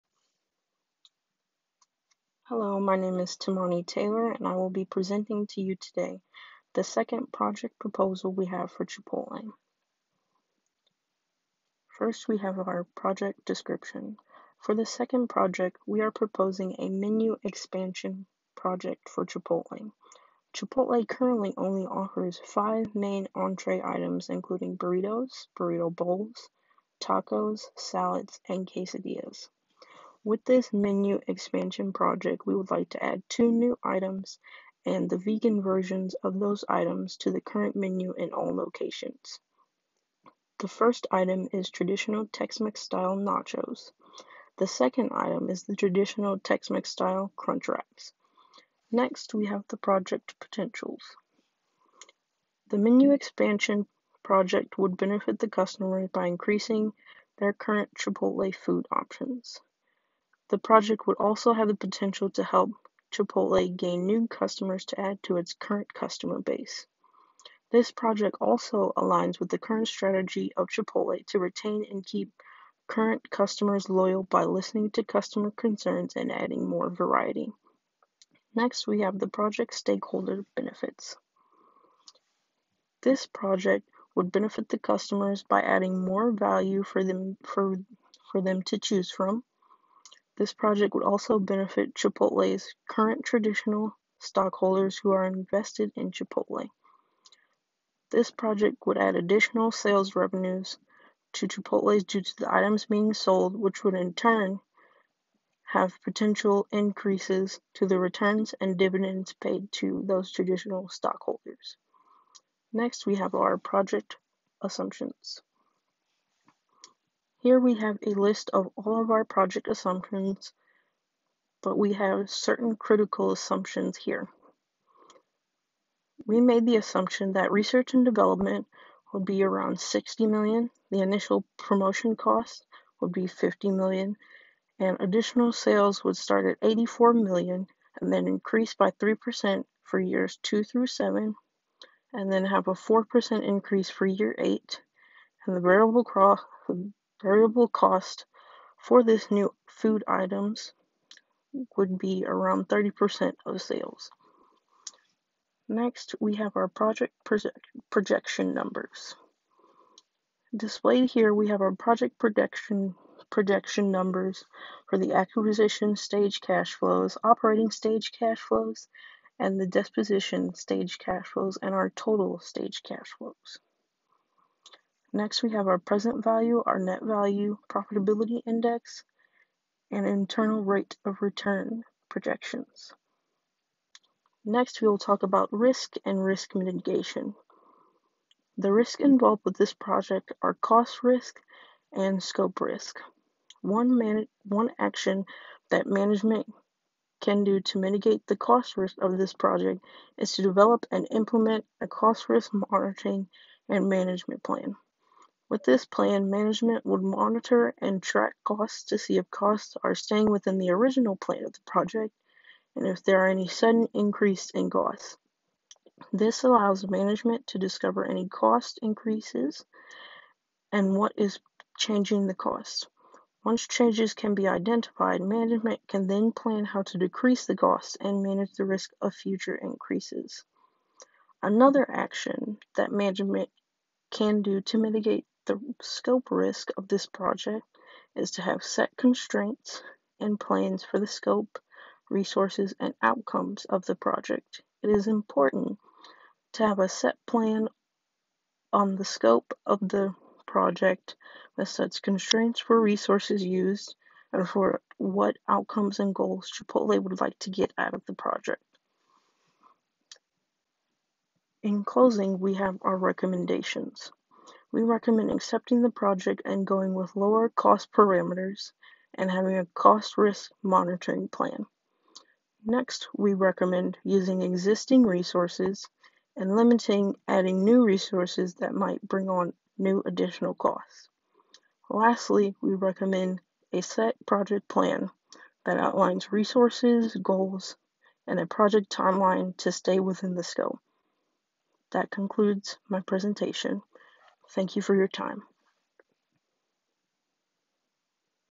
Chipotle Capital Budgeting Project 2 Proposal
Timani: project 2 proposal presentation
Menu expansion project